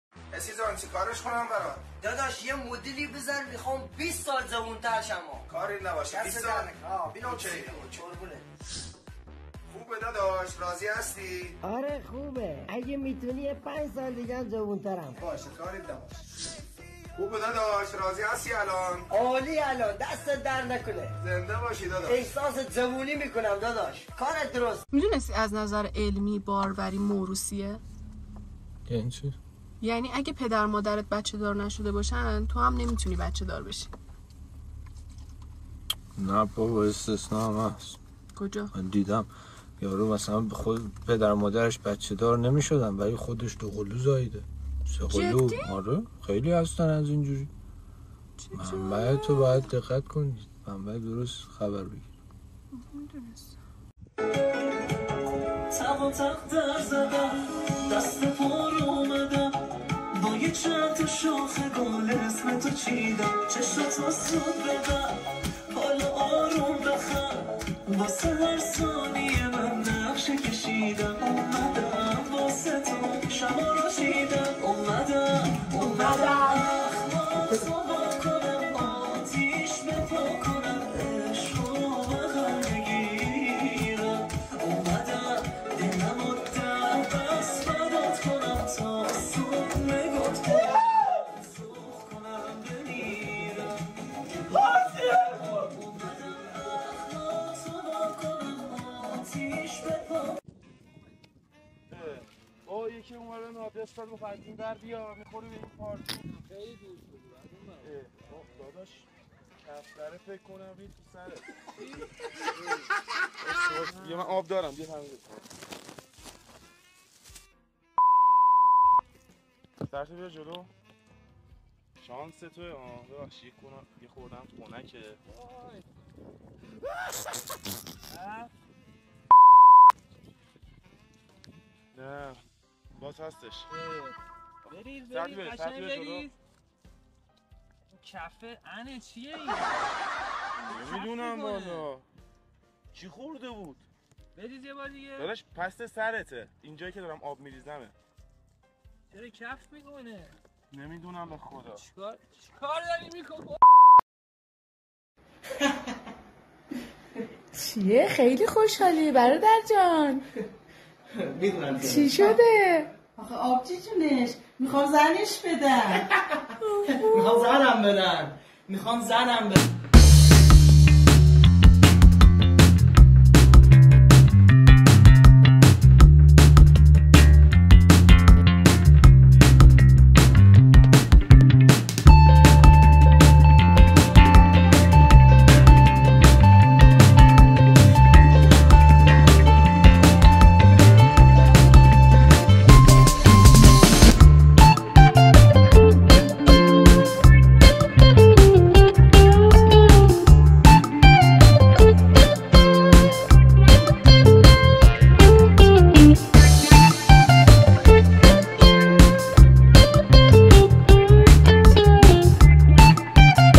Laugh Out Loud with Persian Pranks Part 3 | English Subtitles Included
Get ready to laugh until you cry with the third installment of our Persian funny clips series! With English subtitles for all to enjoy, these hilarious clips are guaranteed to have you in stitches. From pranks to dirty jokes, and even some epic dancing, this video is filled with non-stop laughter. So sit back, relax, and get ready to enjoy and share the fun with your friends!